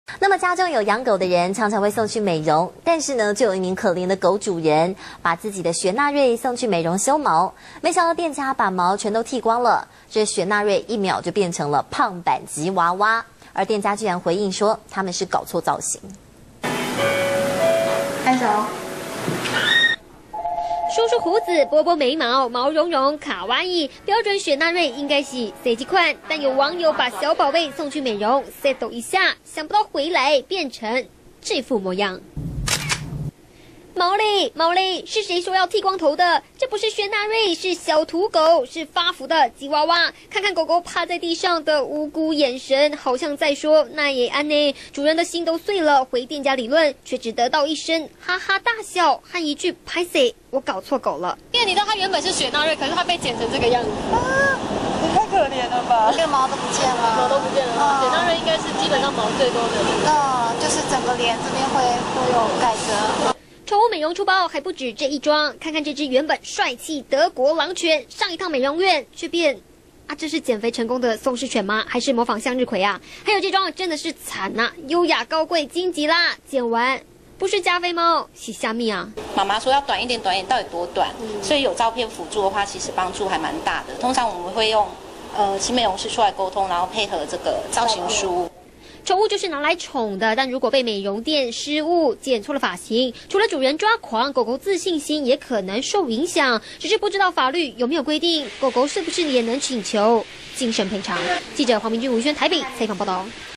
中天新聞》這不是雪納瑞！剃毛一秒變胖版吉娃娃
中天新聞》家中有養狗不定時會送去美容一下，但有一名可憐的狗主人把送「雪納瑞」去美容修毛，沒想到店家把毛全剃光，「雪納瑞」一秒變成胖版吉娃娃，店家竟然回應搞錯狗了「記錯造型」。